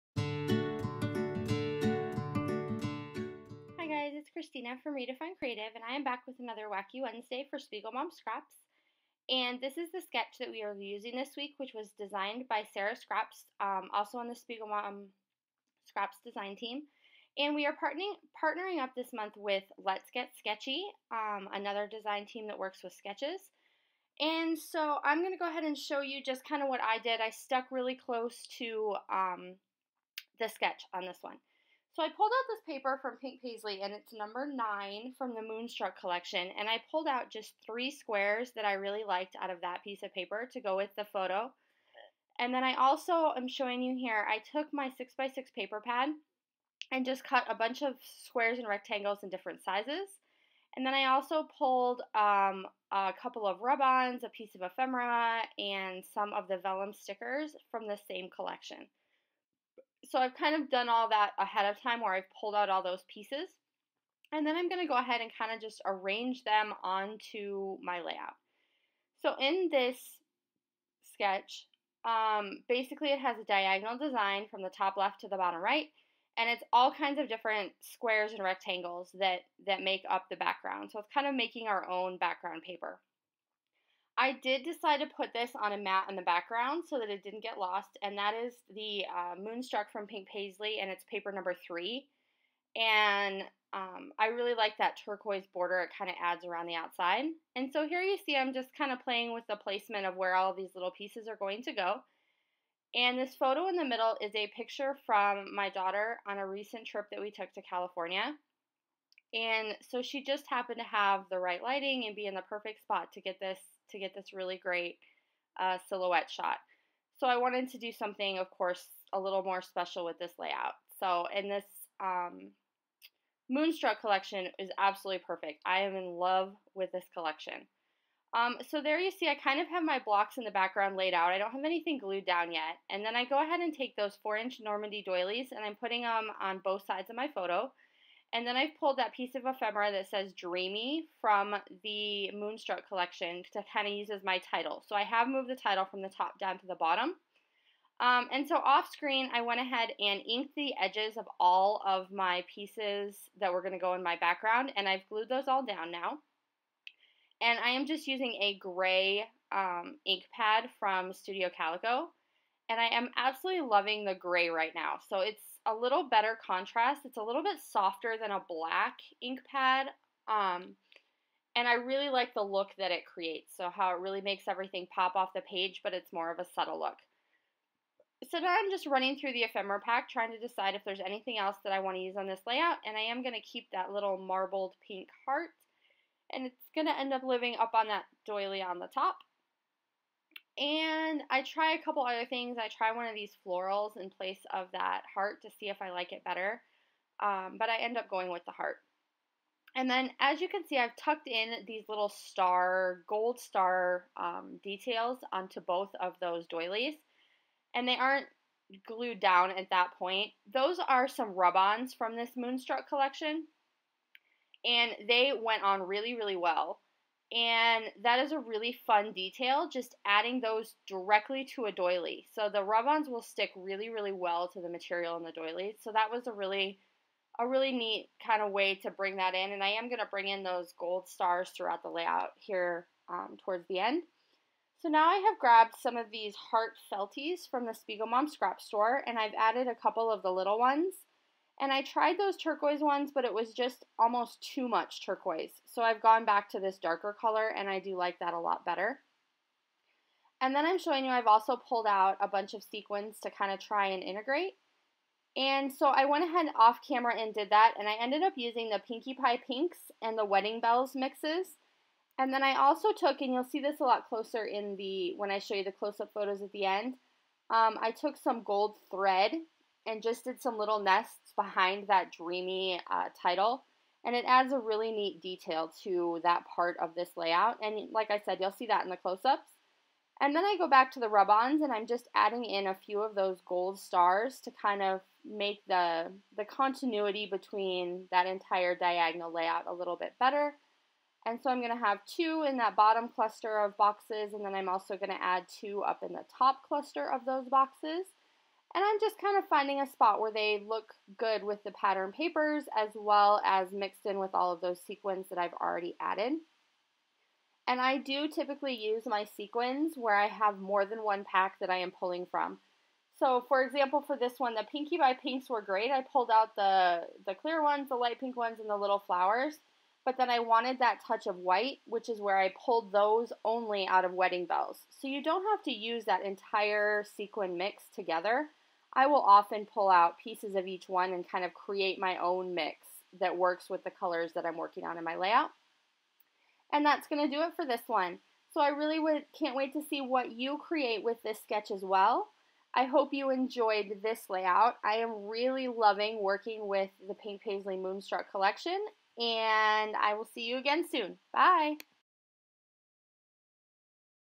SpiegelMom Scraps:Wacky Wednesday Process Video: November Sketch 2: Dreamy
Come on over and check out my take on sketch #2 from this month's collaboration between SpiegelMom Scraps and Let's Get Sketchy! I am absolutely in love with the Pink Paislee Moonstruck collection.

From the SpiegelMom Scraps store, I used: normandy doilies and two sequin mixes: Wedding Bells and Pinky Pie Pinks.